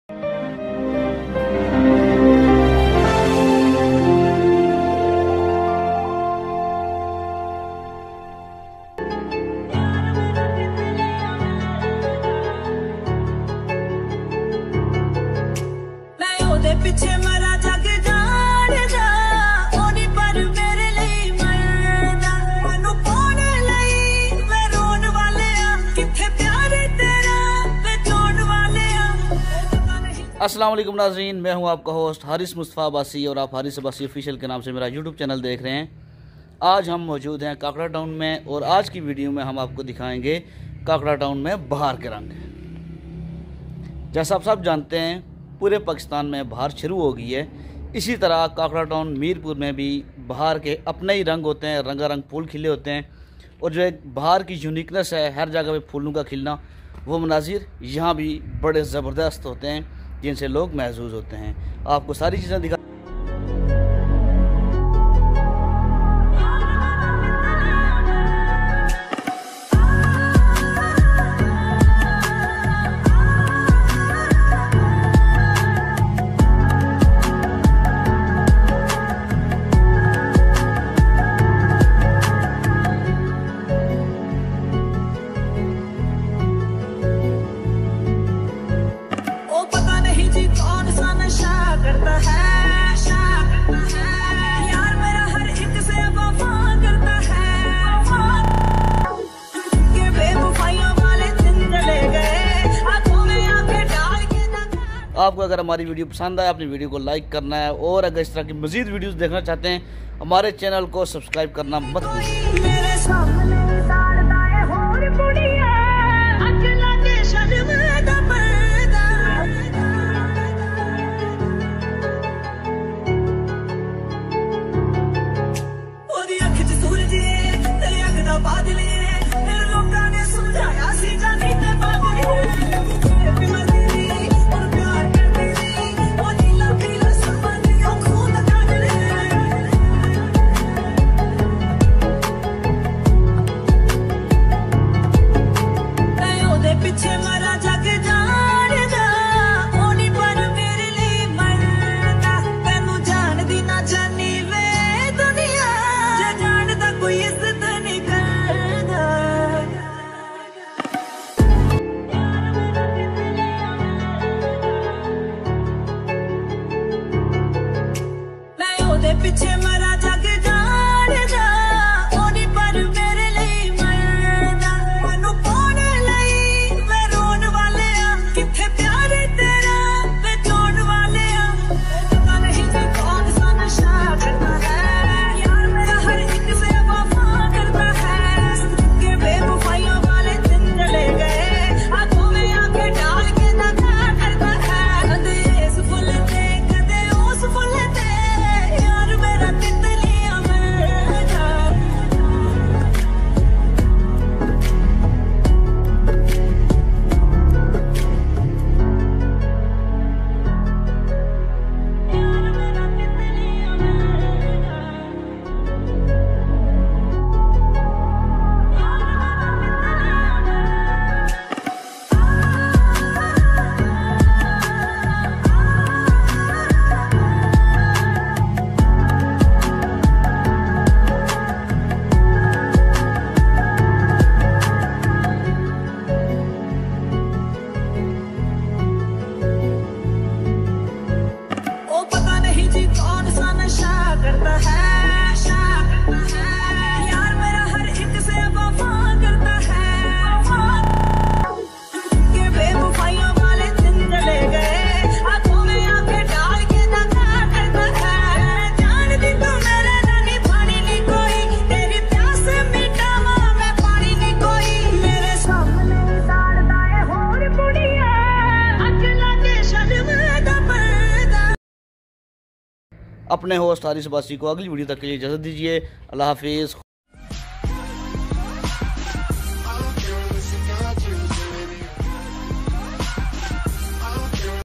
Beauty of kashmir | Kakra town | Mir Pur | Azad Kashmir | ajk beauty | Kashmir vlogs
this video is about beauty of kakra town mir pur Azad kashmir

kashmir beauty
kakra town
mir pur
Azad kashmir
kashmir weather